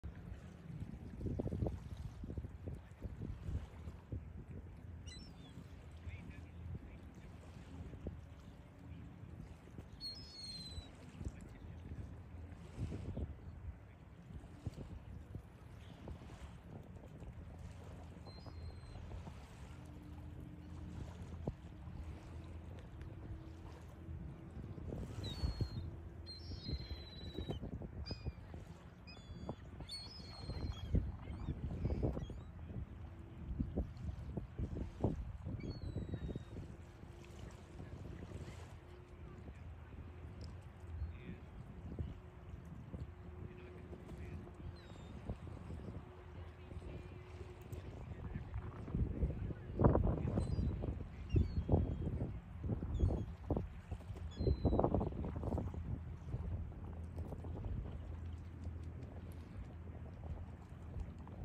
The Gulls Morning Hangout
Morning hangout for the Gulls along the Pensacola  Bay near the gate of Fort Pickens in Florida. Captured with the iPhone 15 Pro Max.